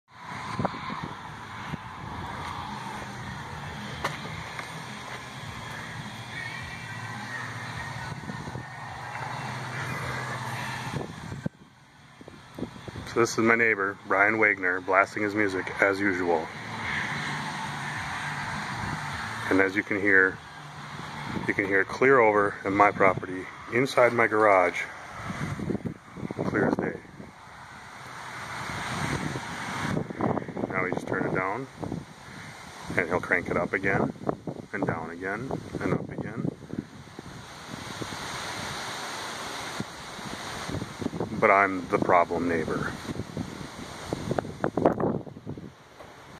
Neighbor from Hell: Episode 3
An example of his normal music volume. But if I did that, there'd be some other petty stunt he'd pull to harass me. Something would be dumped in my yard again.